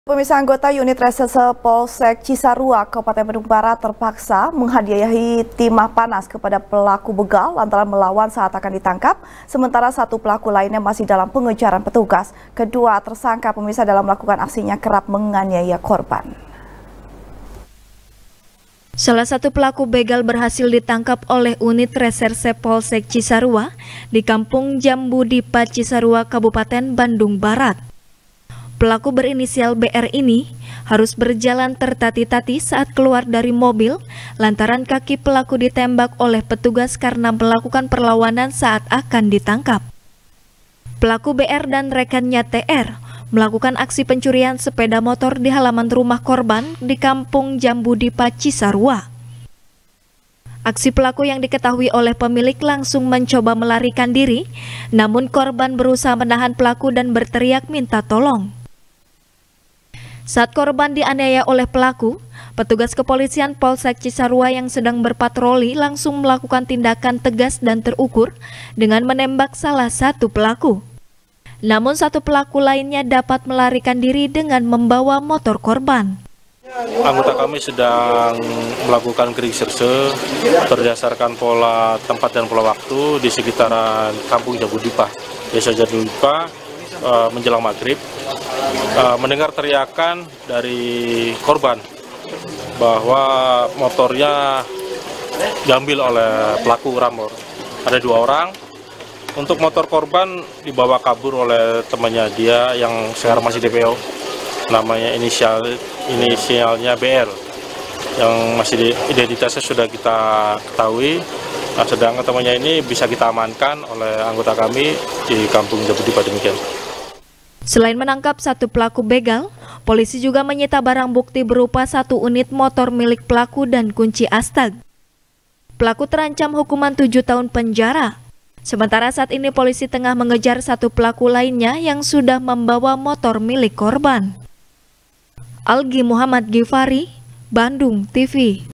Polisi Tembak Pelaku Begal | SBR | BANDUNGTV
SBR | BANDUNGTV | FARHAT MUMTAZ | Anggota Unit Reserse Polsek Cisarua,Kabupaten Bandung Barat,Terpaksa Menghadiahi Timah Panas Kepada Pelaku Begal,Lantaran Melawan Saat Akan Ditangkap,Sementara Satu Pelaku Lainnya Masih Dalam Pengejaran Petugas.Kedua Teresangka Dalam Melakukan Aksinya,Kerap Menganiaya Korbannya.